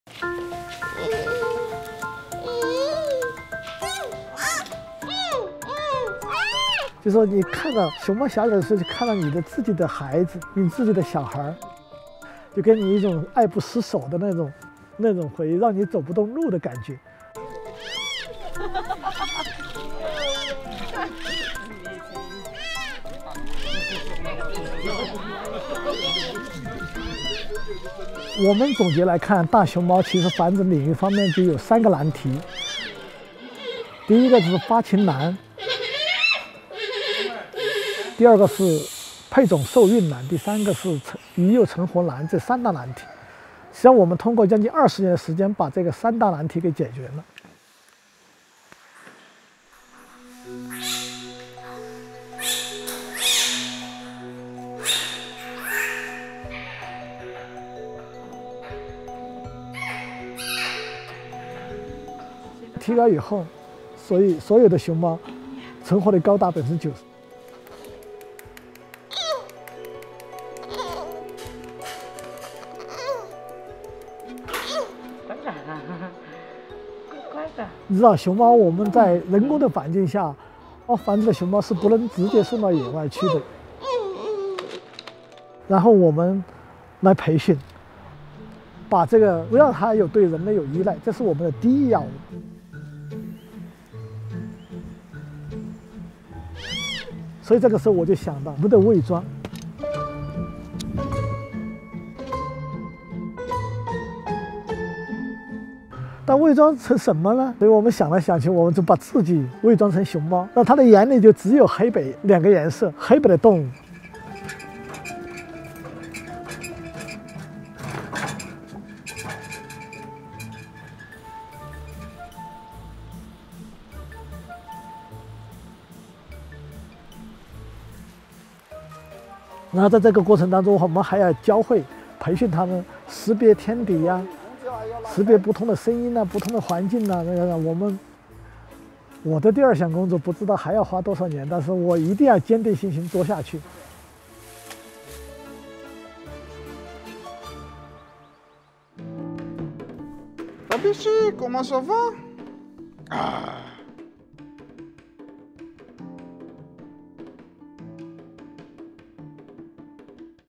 Raising Cute Pandas: It's Complicated | National Geographic
Giant panda cubs are adorable fluff balls that squeak and squeal. This endangered species is also incredibly tricky to breed and raise in captivity. In the 1960s, only 30 percent of infant pandas born at breeding centers survived. Today 90 percent survive. So, what changed?

In the last 20 years, China has successfully tackled three of the biggest problems holding the giant panda back. Through research and experimentation, researchers at China's breeding centers have discovered how to encourage captive pandas to mate, how to make sure the pregnancy is successful, and how to keep the panda cubs alive once they've been born. For mating, they found that offering the females a choice of mate as well as enriching panda diets leads to a better chance of successful mating. A panda pregnancy can range from 73 days to 324 days depending on the weight of the mother panda and conditions she experiences during pregnancy. This knowledge, backed by medical advancements in artificial insemination, has allowed for more successful pregnancies in captive pandas.

Pandas often give birth to twins, and mothers usually choose to nurture only the stronger cub, resulting in the other twin's death. Panda keepers have found a way to save this cub by "sharing custody." Periodically, the keepers will swap out the infants, caring for one themselves while the other is with the mother. This way, every cub has a chance to survive. When in human care, the keepers must frequently massage the gut of the cub to help stimulate bowel movements—as infant pandas are unable to defecate without assistance.

All of these factors have led to the spike in the panda's survival rates. More recently, the China Conservation and Research Center for the Giant Panda has made another breakthrough: It’s the only center in the world to successfully breed pandas and then release them into the wild. Five pandas have been released since 2006, though two have died. Before release, the pandas must go through a series of trials meant to test their abilities to survive in the wild, while avoiding human interference as much as possible. China's dedication to panda research is leading the rest of the globe in panda conservation.